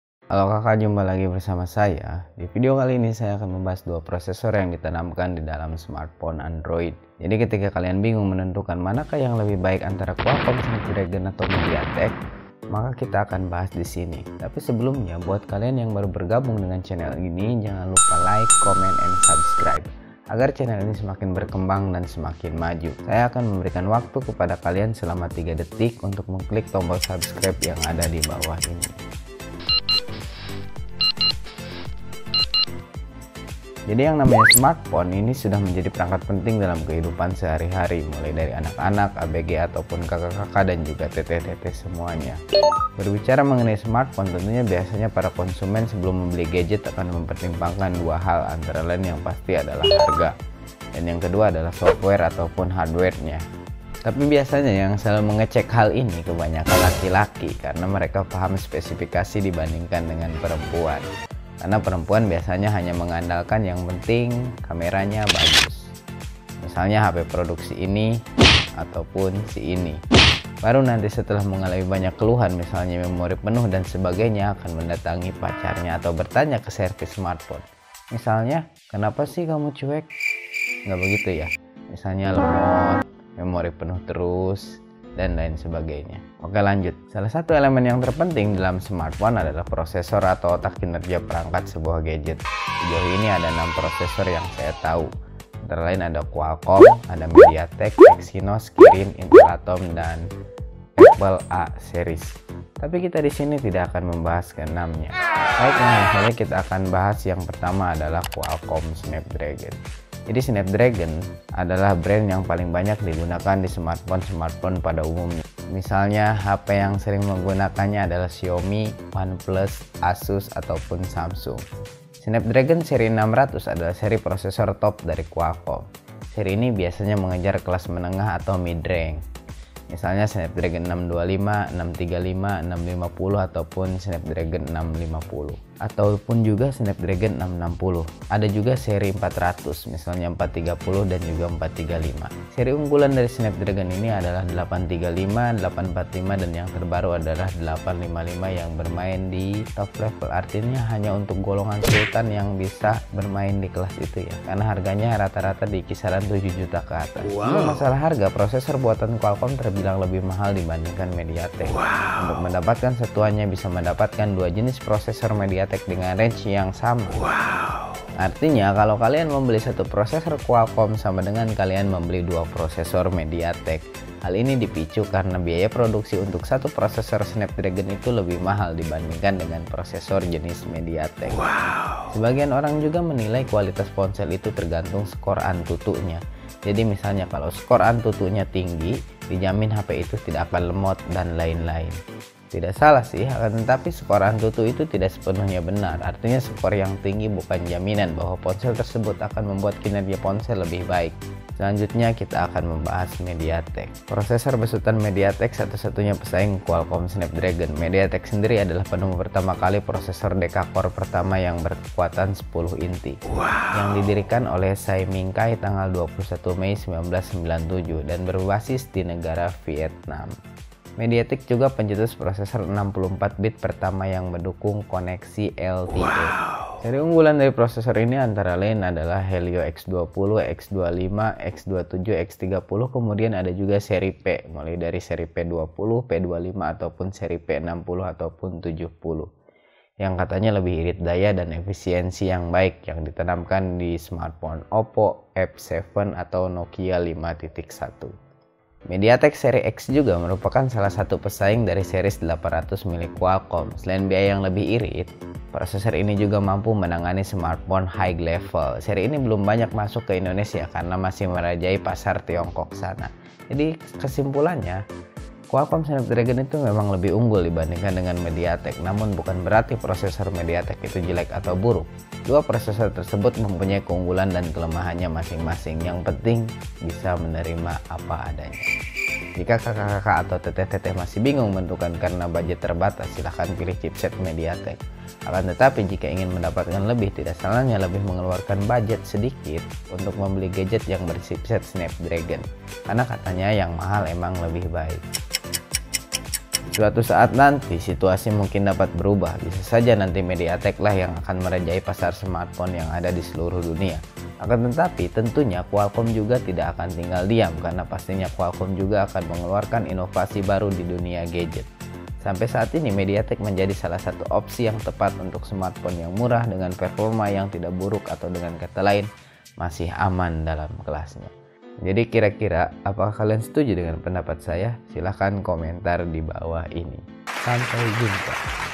Qualqomm Snapdragon vs Mediatek! Pilih yang Mana Yah?
Persaingan antara Qualqomm vs mediatek sudah bukan jadi rahasia umum lagi
Apakah Qualqomm lebih Bagus daripada Mediatek?